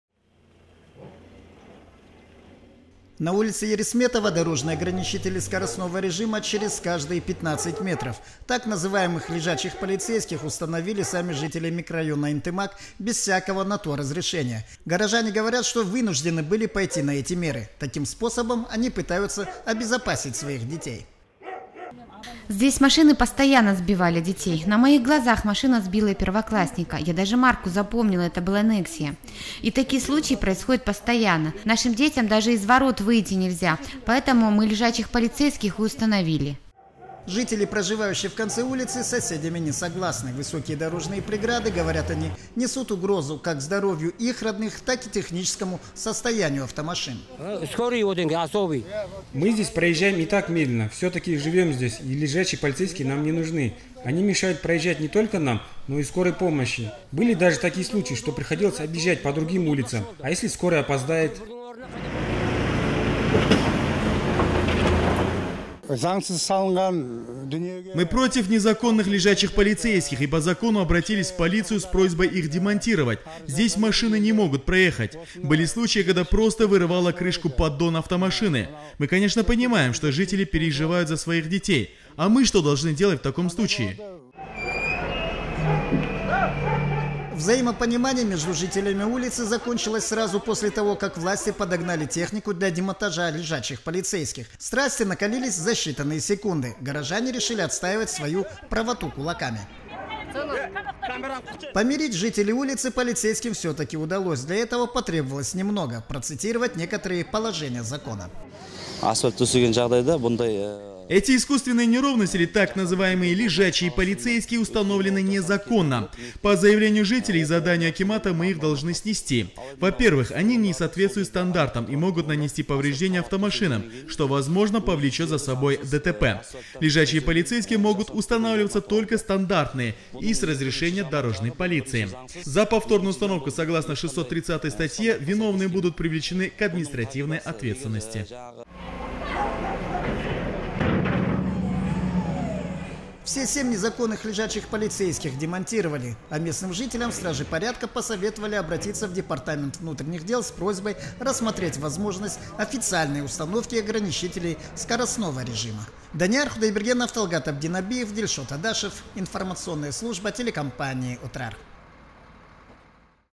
"Лежачие полицейские" рассорили соседей
Конфликтом между жителями микрорайона "Ынтымак" закончилась попытка городских властей демонтировать незаконные "лежачие полицейские" на одной из улиц микрорайона. Жители улицы Ерисметова разделились на два противоборствующих лагеря. Мнения - нужны ли на дороге искусственные неровности, ограничивающие скорость движения автомашин, - были противоположные, и каждый из жителей защищал собственную безопасность и безопасность своих родных.